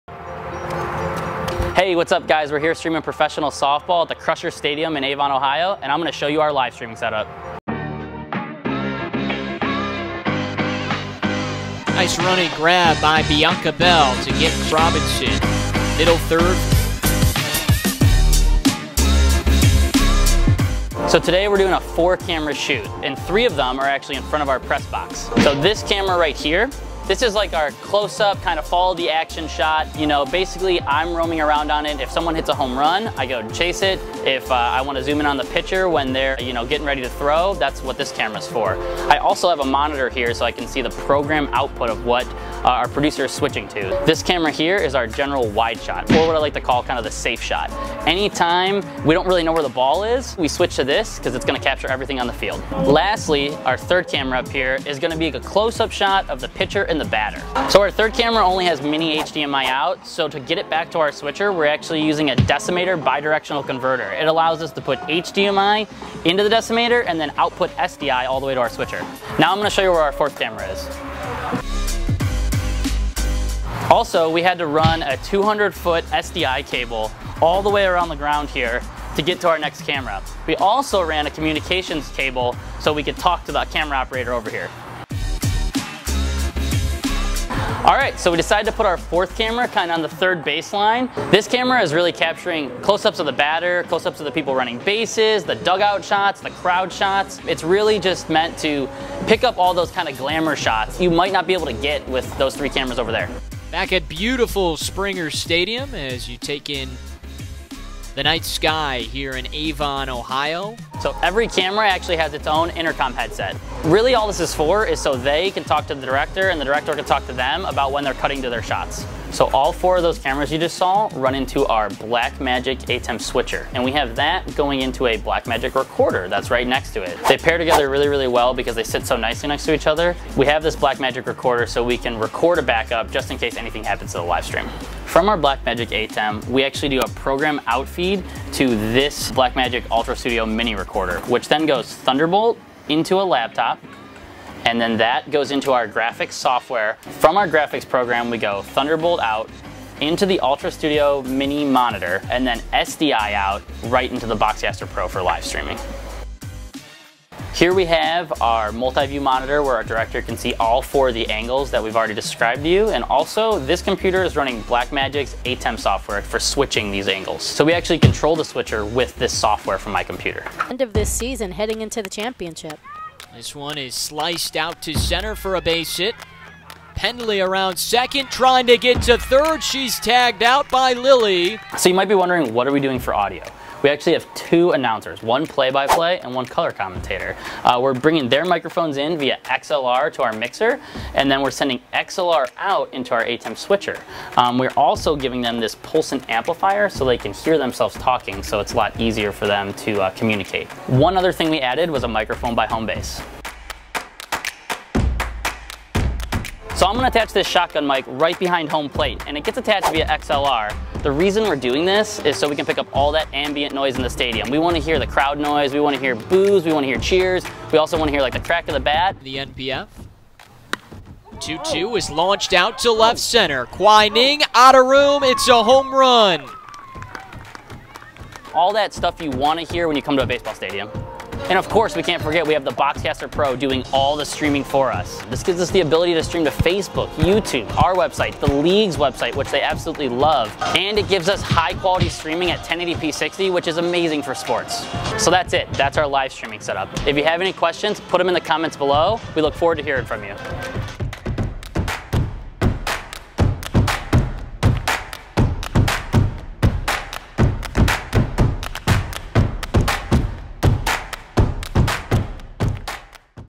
How to livestream a Softball or Baseball Game
The equipment we used for this project:

Camera 1 and 2

Camera 3 (HDMI)

Camera 4

Blackmagic ATEM Switcher

Blackmagic Recorder

BoxCaster Pro

Broadcaster Headset

Headphone Amplifier

Audio Mixer

Intercom System

Field Microphone

Capture Card

Playback Device

Field Monitor

Varizoom Controller

BoxCast is a complete, easy-to-use live video streaming solution for organizations.